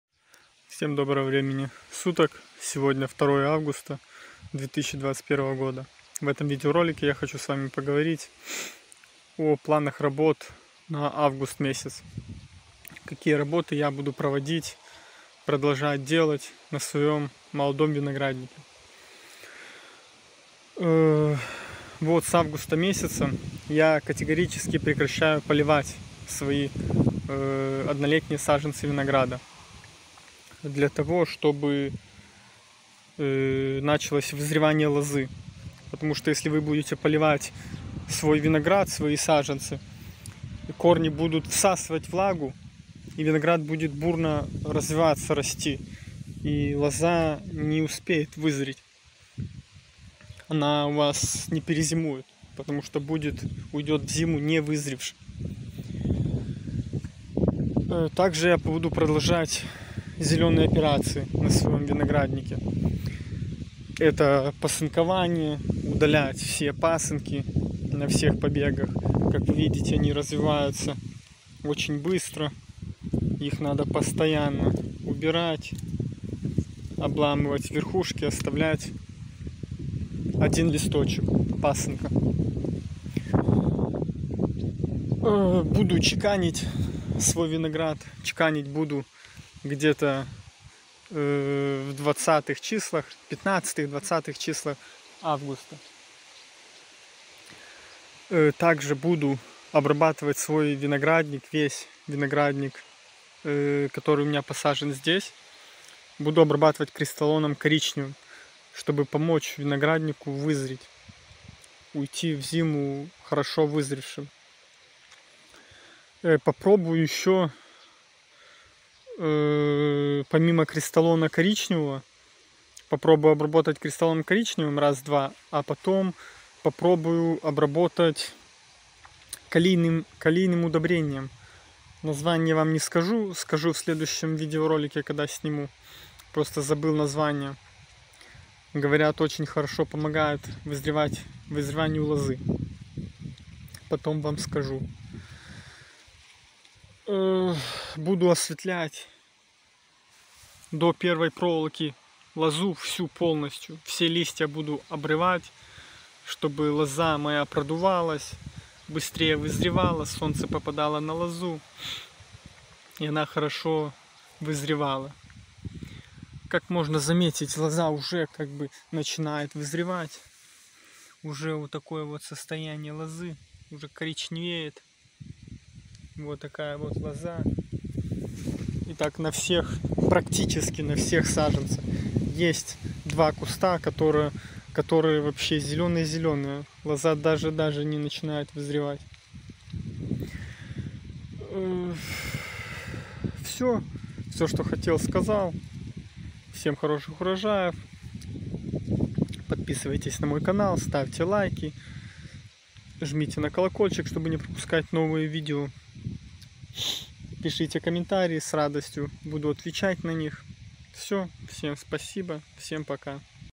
планы работ в августе на однолетних кустах винограда
что нужно делать в августе месяце на винограднике первого года посадки